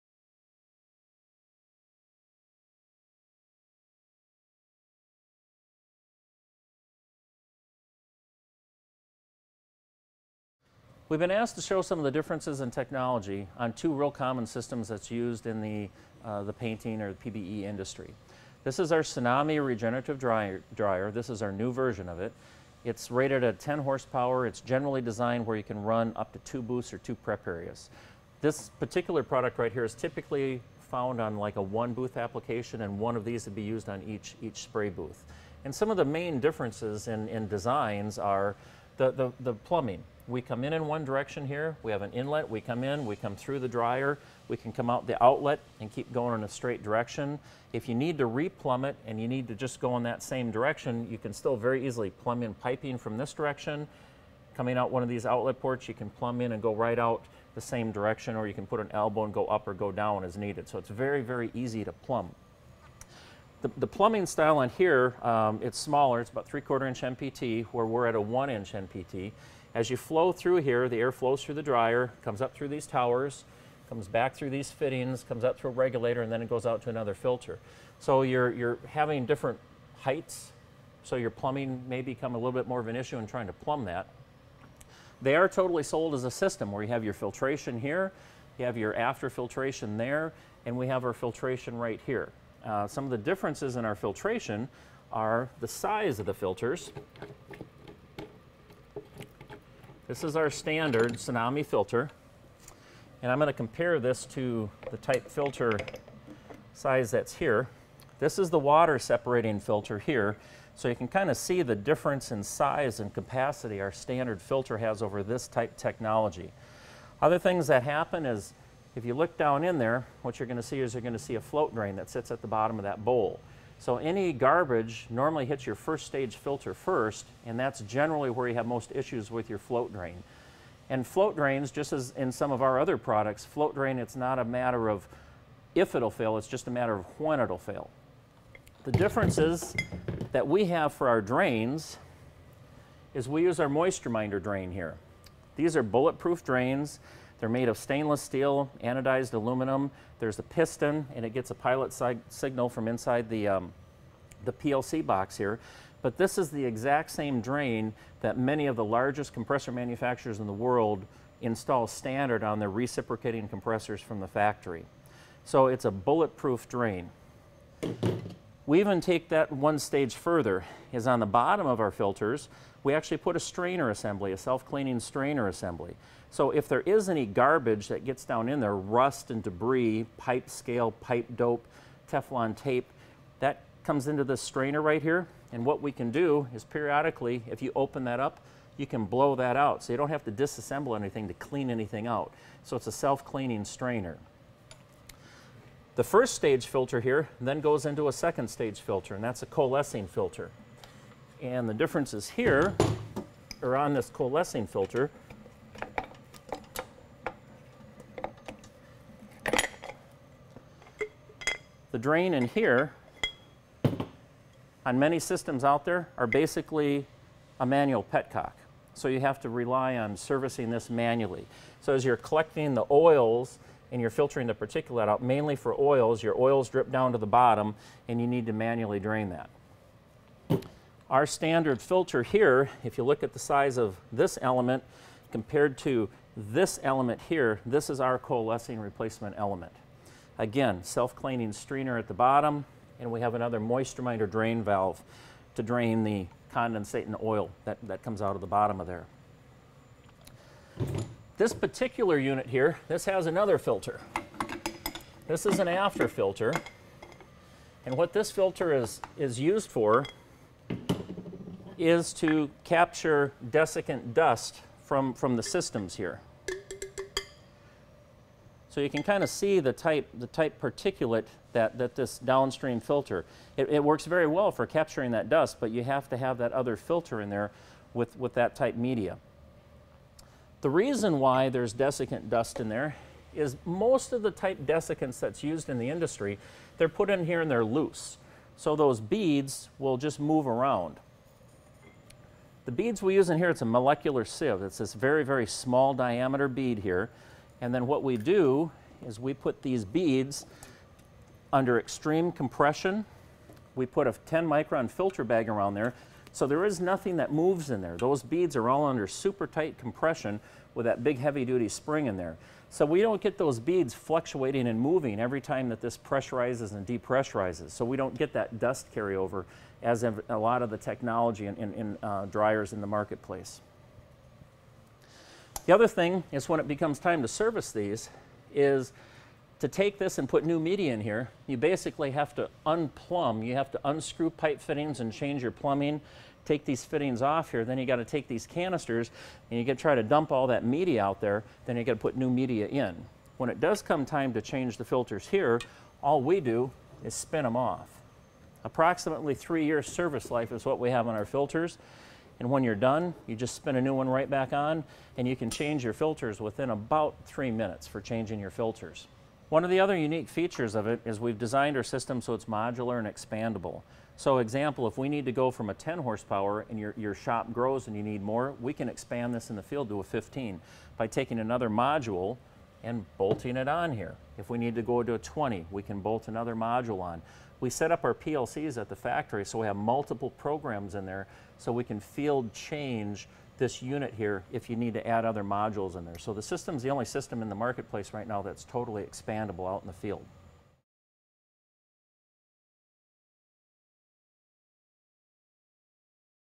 Regenerative Dryer vs. Competition
The differences in technology between a Tsunami Regenerative Dryer and competition.